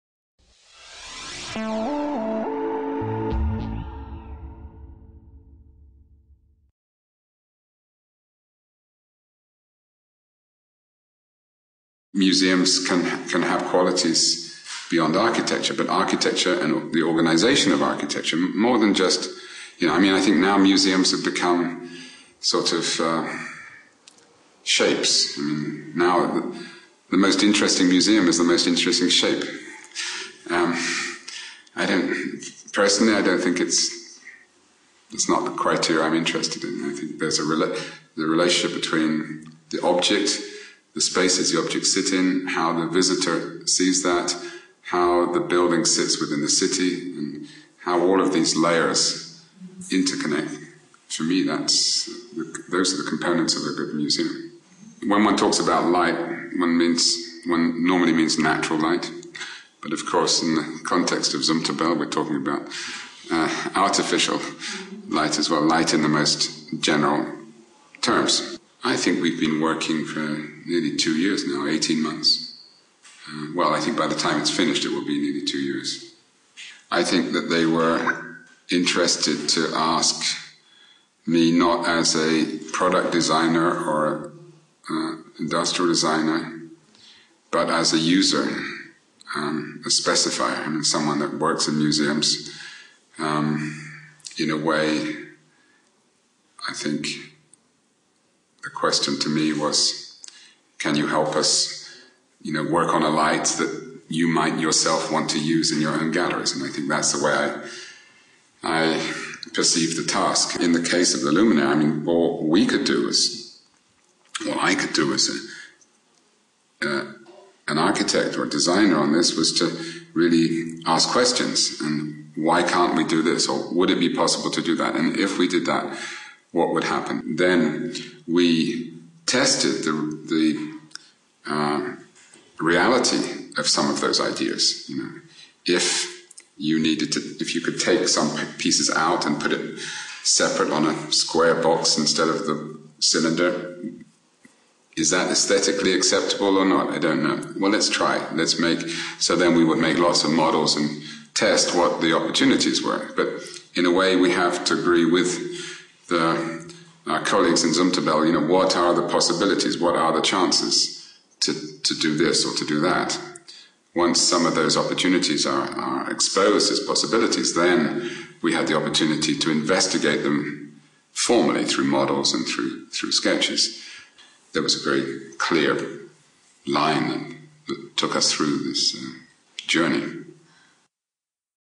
David Chipperfield on museums, architecture and the ARCOS luminaire
The architect David Chipperfield in an interview on museums and architecture and the lighting of museums. He also talks about his collaboration with Zumtobel in the designing and developing of the ARCOS luminaire.